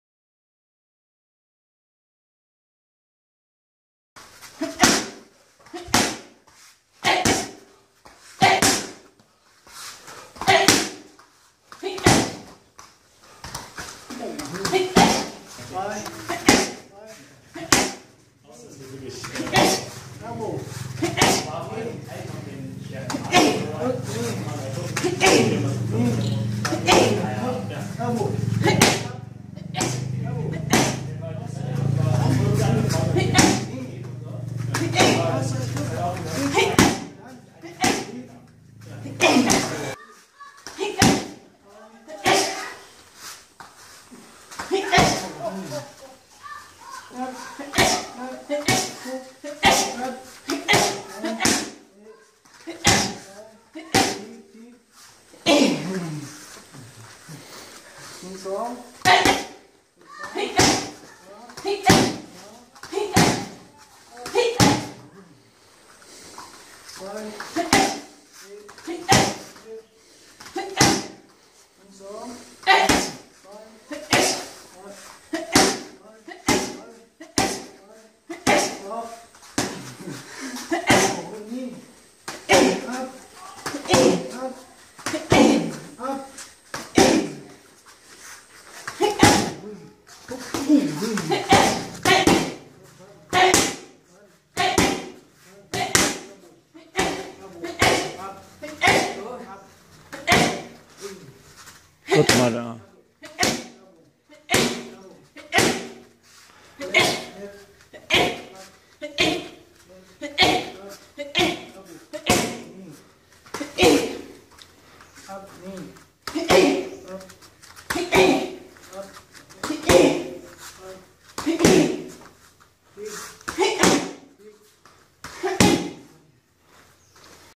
Miriam Sabot Pawork at Sitjemam Muay Thai Pai
Miriam Sabot (ITA)  is back on the pads with Kru Chet  at Sitjemam Muay Thai Camp in Pai, just 3 days after scoring a big win In Kuala Lumpur  and becoming the new Z1 WMC world Champion while representing Muay Farang Team.